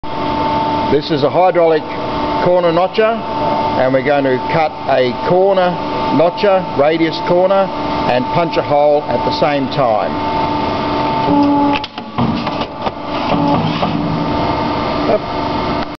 Shaw Cut Radius Corner Notcher
Does Round Corners and can punch a hole at same time if required.  Double Ended, other end is standard Corner Notcher. 4 different Radius's and 2 different punch sizes
Hydraulic Sheetmetal Machine